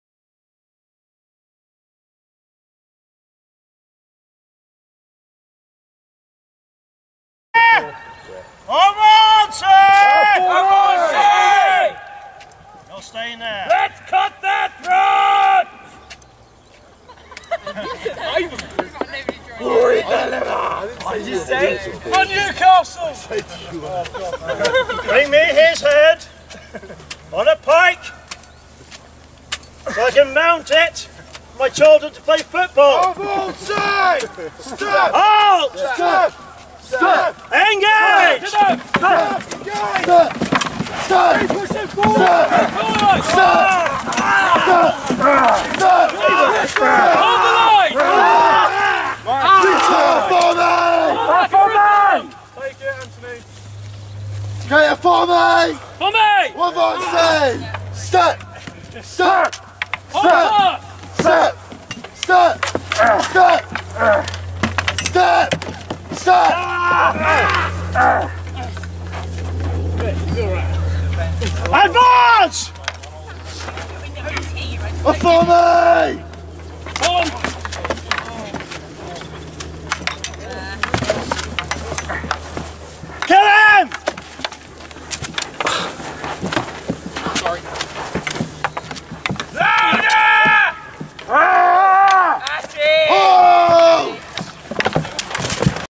Historia Normannis Winchester - Spear vs Lance Shield Wall 3
A video taken for training purposes, of our Winchester Men-at-Arms practicing their Shield wall tactics. In this fight, we have one shield wall of Short arms and Lances, and in the attacking side, two-handed spears.

Shield walls were how most battles would have been fought during the 12th century and even earlier, where both armies had matching lines or, "walls" of shields facing one another. Insults would be shouted, projectile weapons would be thrown and you would eventually be in close quarter combat. The Shield wall was a very nasty and dangerous place to be.